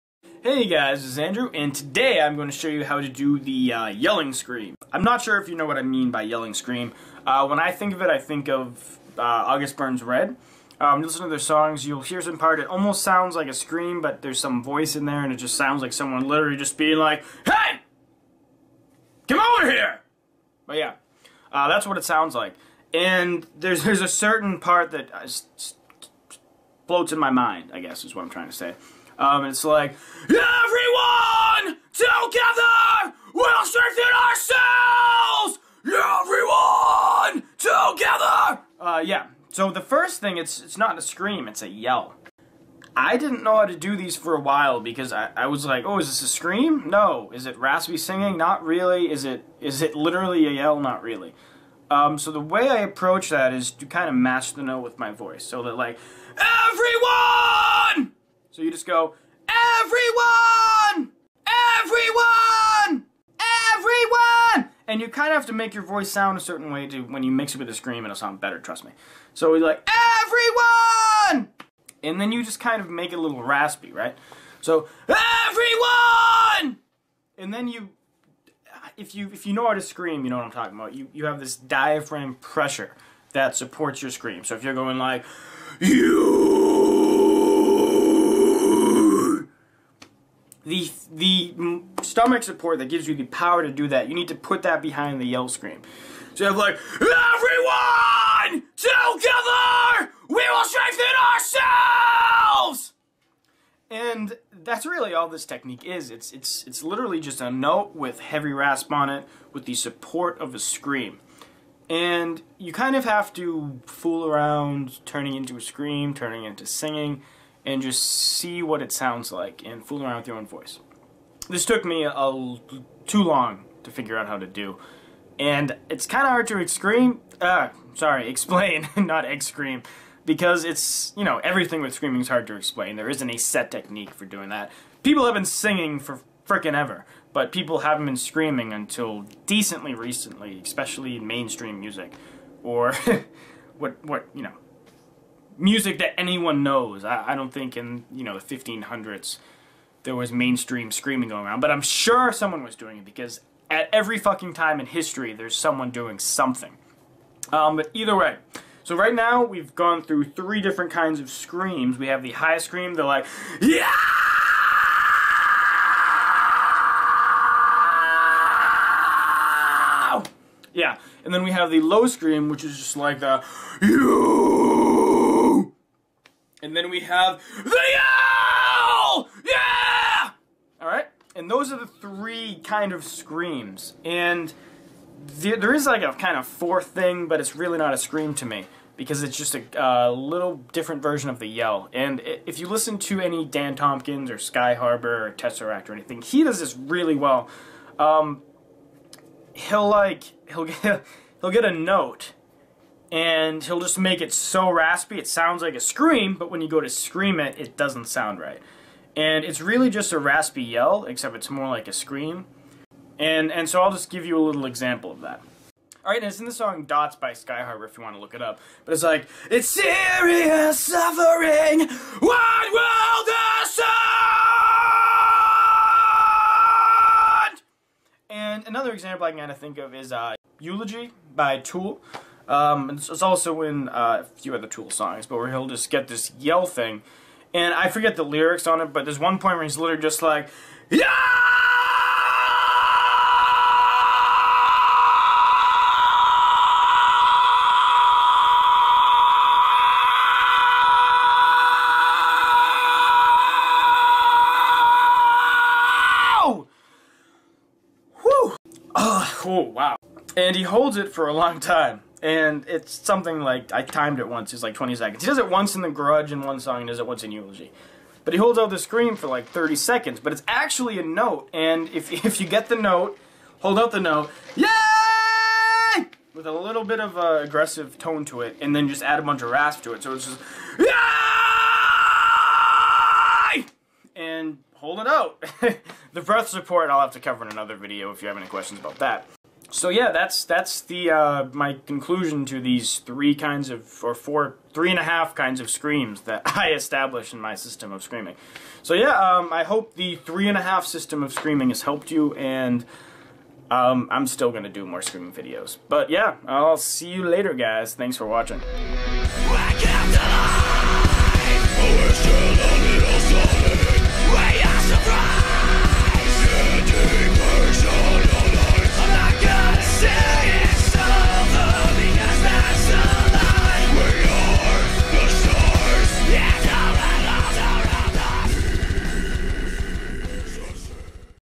Screaming Lesson #3: Yell Screams
This is part 3 of my screaming lesson series! In this video I cover what I call 'yell screams'.  You have to match the pitch and then add rasp and 'pressure' to get something that sounds like a yell, but also like a scream. I also cover how to apply this kind of scream to something that sounds completely different. In the next screaming lesson we will cover what I call the 'moan' scream. When I run out of types of screams i'm going to cover screaming exercises and song applications.

Screaming lesson by Andrew Southworth